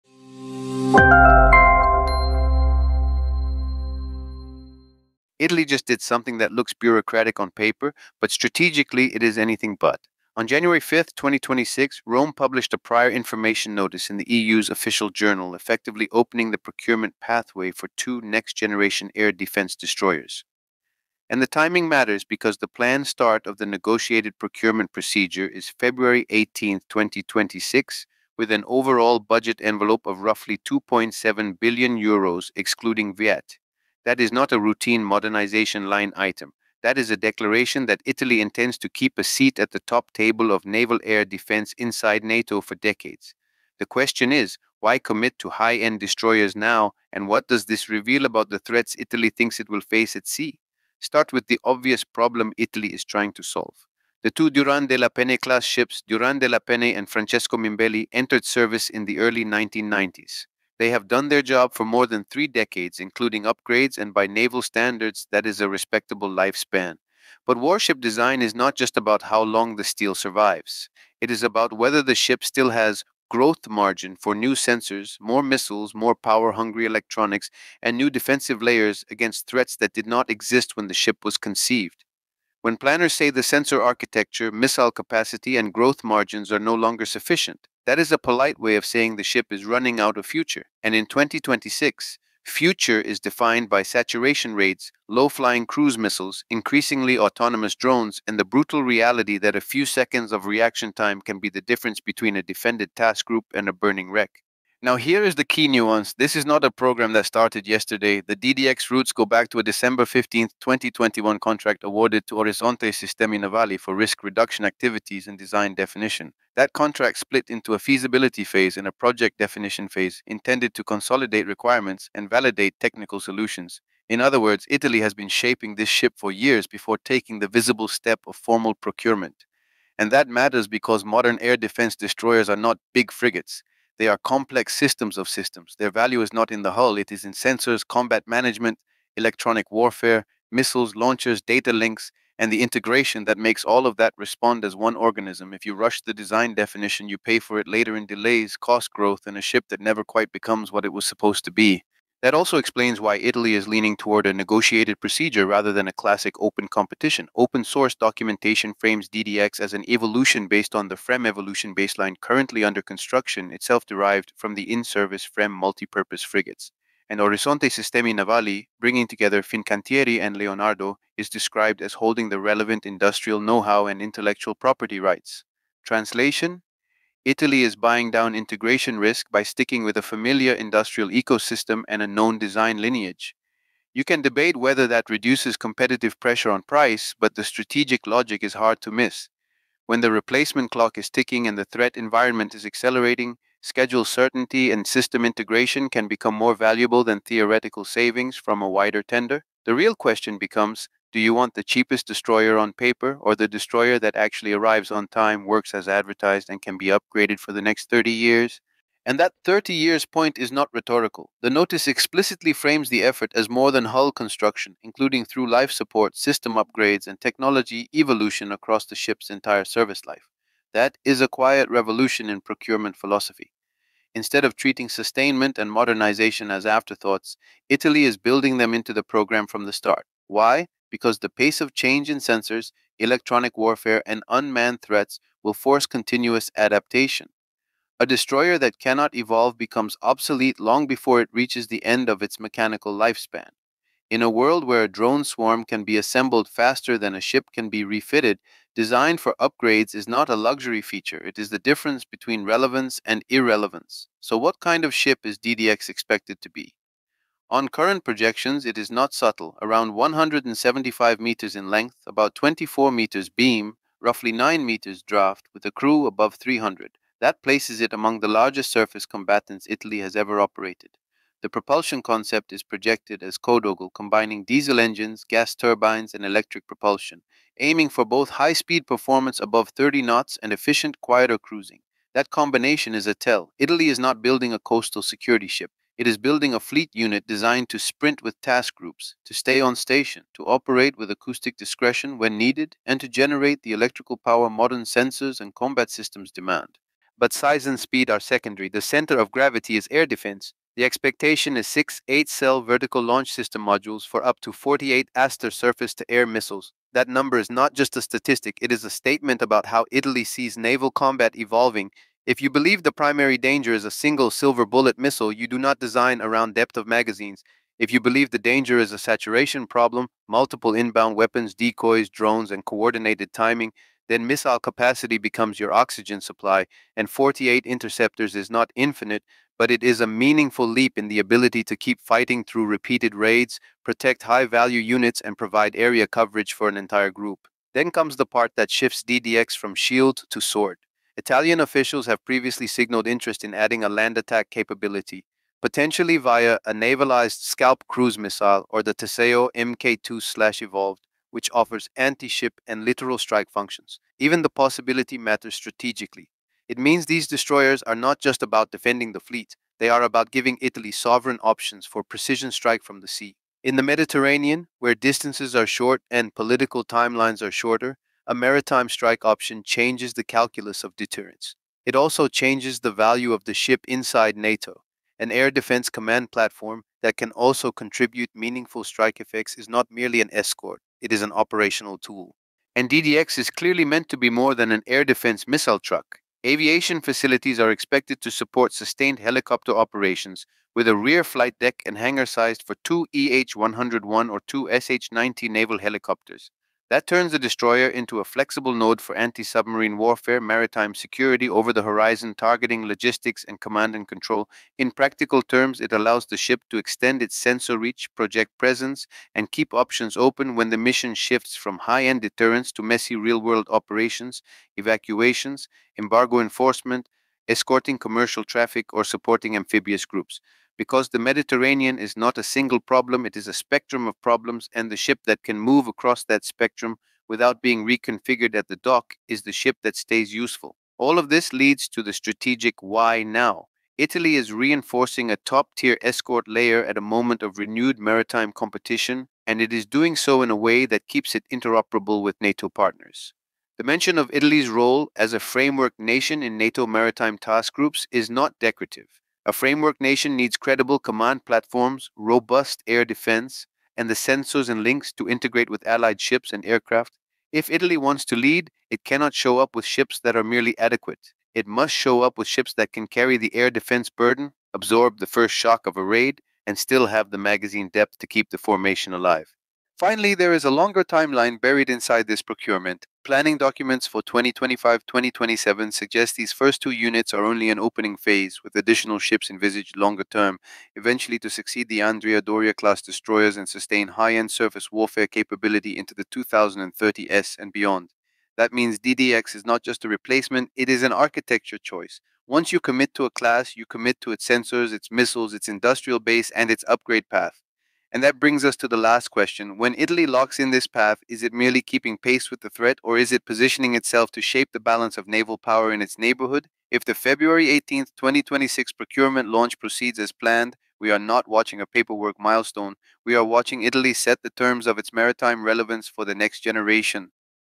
Italy’s DDX Destroyers: The €2.7B Air-Defense Upgrade NATO Will Rely On
Italy has officially opened the procurement pathway for two next-generation air-defense destroyers—often referred to as the DDX program—after publishing a prior information notice in the EU’s Official Journal on January 5, 2026, with a negotiated procedure expected to begin on February 18, 2026. Backed by an estimated €2.7 billion budget envelope (excluding VAT), these ships are designed to replace the Durand de la Penne-class and secure Italy’s top-tier naval air-defense role inside NATO maritime task groups.

In this video, we break down what Italy is really buying: a large, future-ready surface combatant expected to carry up to 48 Aster surface-to-air missiles in multiple VLS modules, built around a modern sensor and combat-management architecture with long-term through-life upgrades. We also explore why Italy is pursuing a negotiated procurement path through Orizzonte Sistemi Navali (Fincantieri + Leonardo), how the design lineage links to FREMM Evolution, and what this means for Mediterranean deterrence, fleet protection against saturation attacks, and NATO interoperability.

Finally, we assess the strategic implications of possible land-attack and littoral strike options, the role of helicopters in multi-mission tasking, and what this program signals about Europe’s evolving naval power competition.